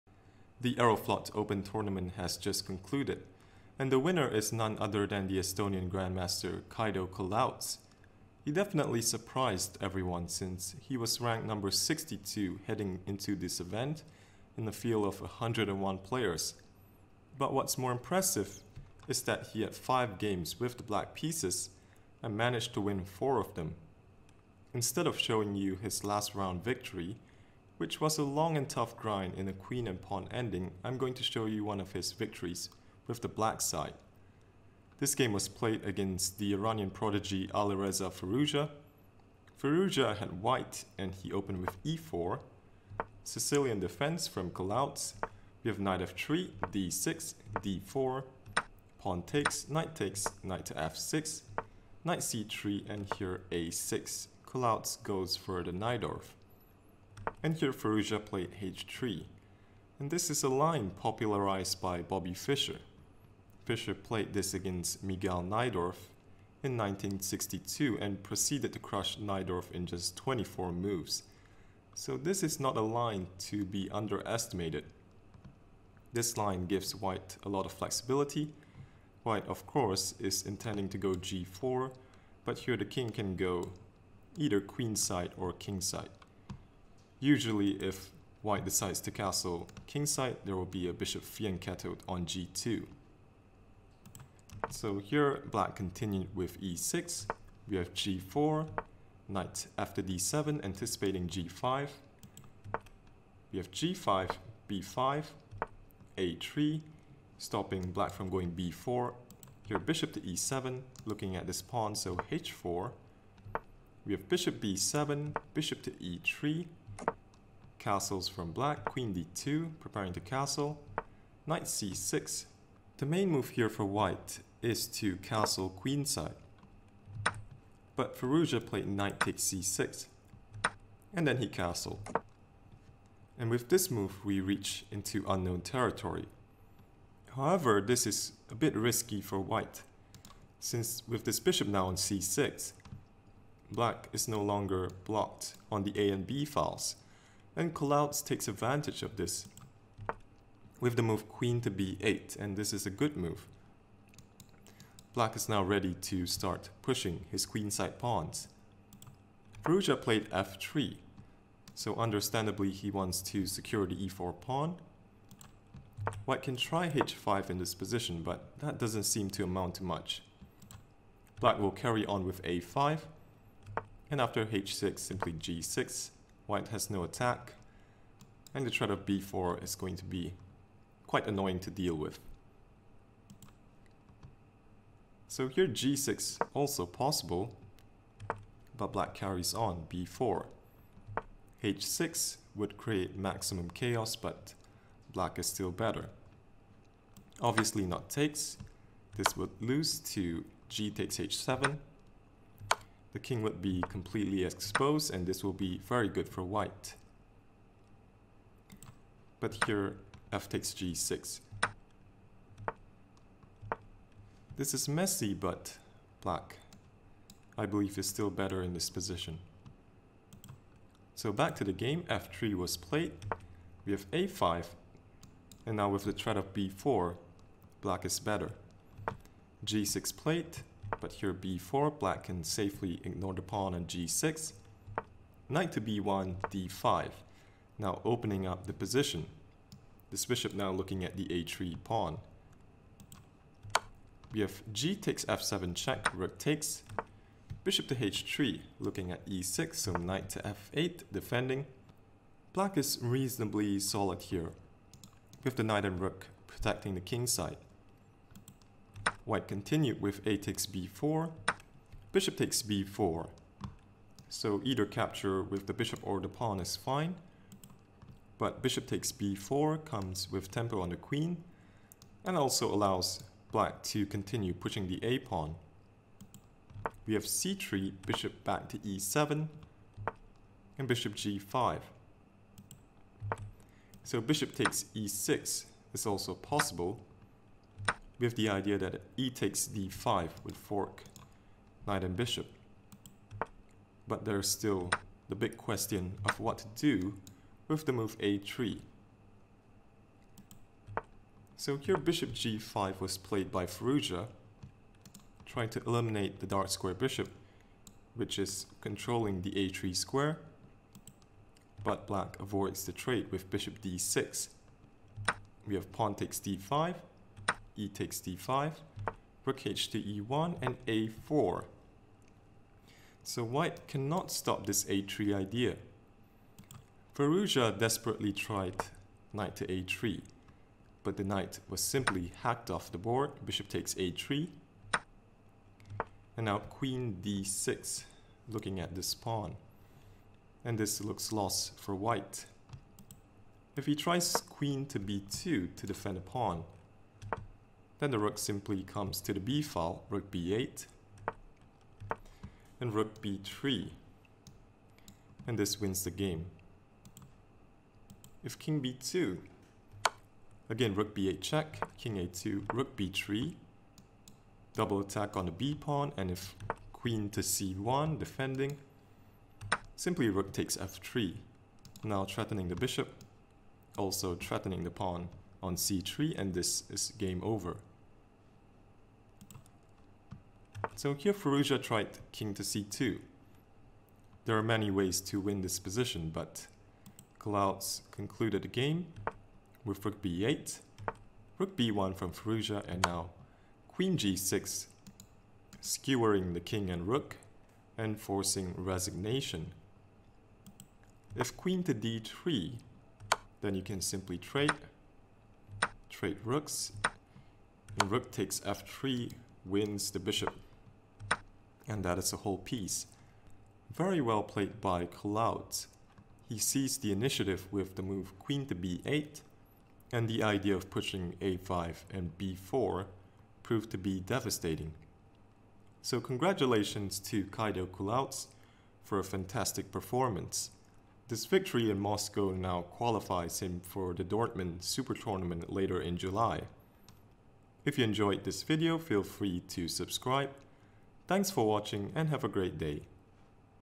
Kaido Kulaots is the Aeroflot Open 2019 Champion!
The Aeroflot Open has concluded and the winner is Estonian Grandmaster Kaido Kulaots. Kulaots was one of the underdogs in this event, ranked at the bottom half of the field but he scored an amazing 7/9. But what's more impressive is that he had 5 games with black and managed to win 4 of them. Instead of showing his last round victory, which was a long grind in a queen and pawn ending, I'm going to show you one of his victories with the black side.